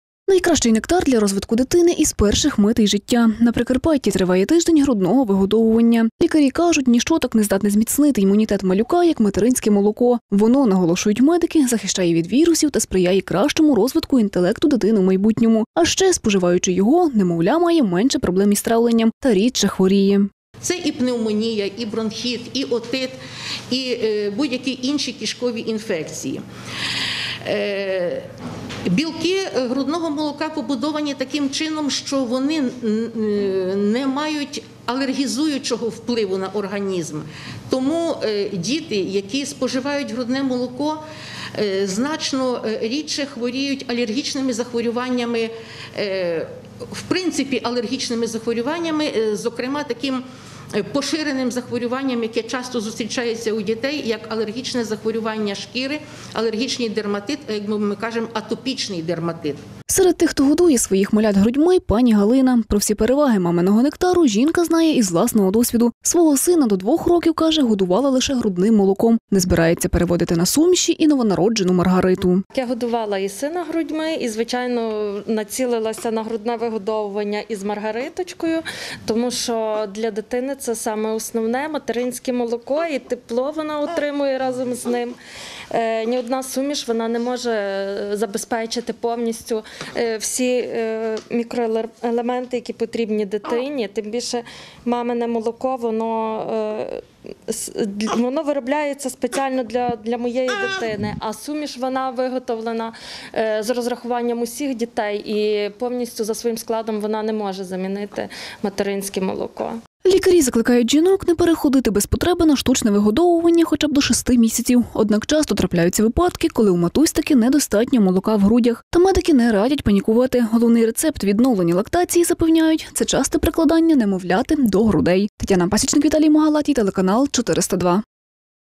На Прикарпатті триває тиждень підтримки грудного вигодовування
Лікарі кажуть: ніщо так не здатне зміцнити імунітет малюка, як материнське молоко.